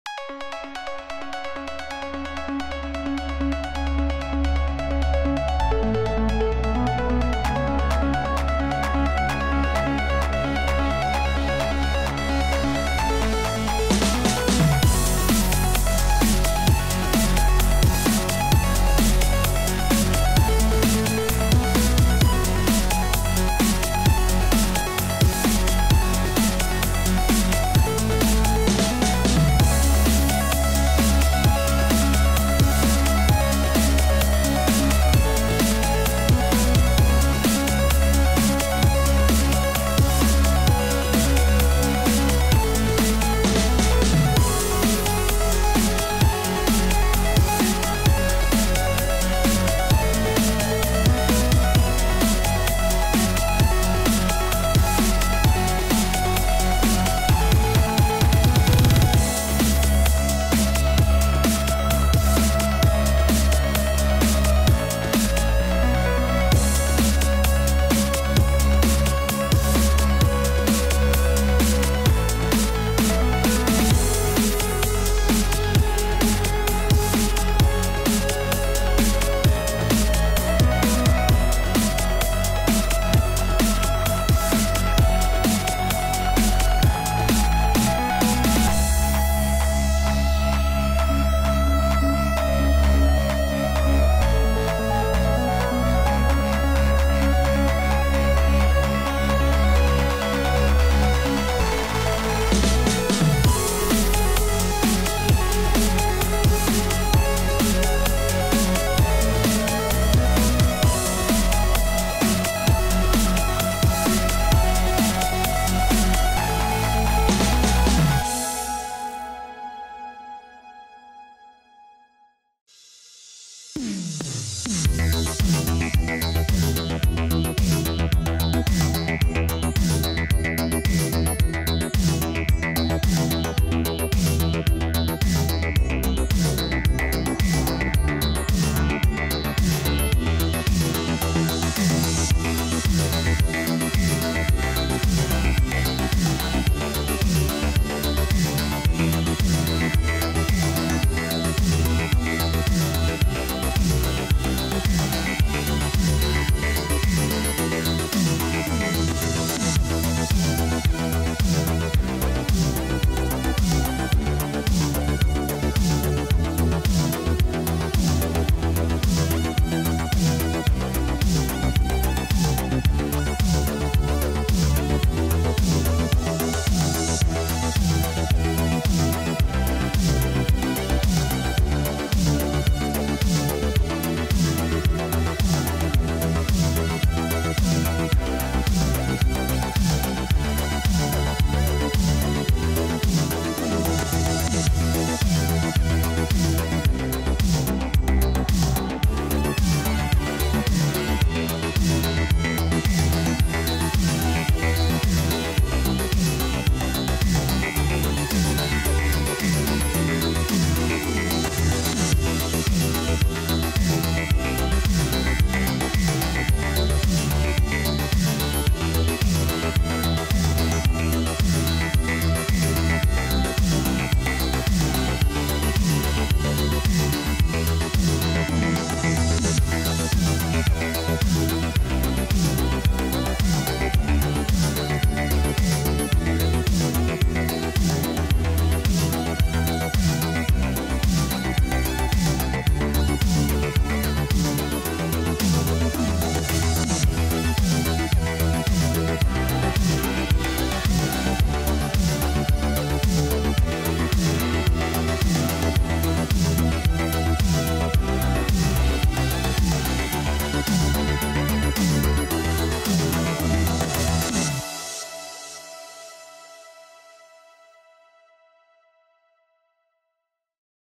Studio Baku Showreel 2024
A little montage of my animation journey so far here on youtube.